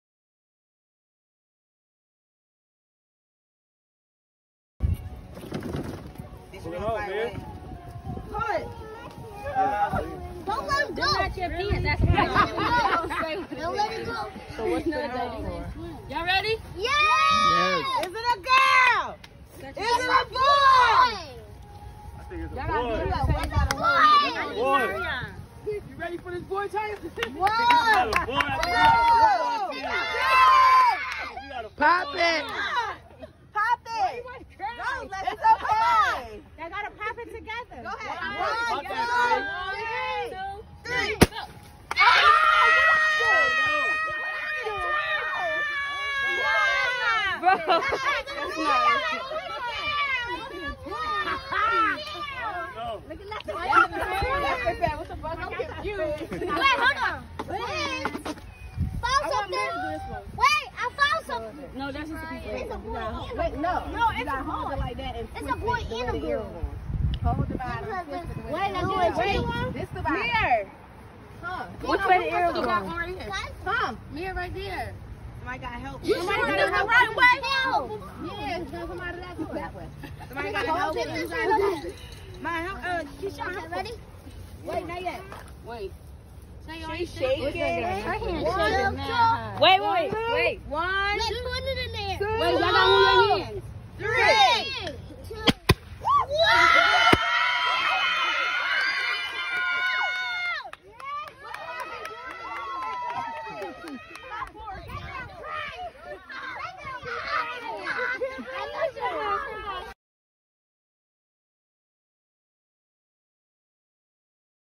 GENDER REVEAL | What Will It Be ?💙💖
In this video you will watch our gender reveal I hope you guys enjoy. 💗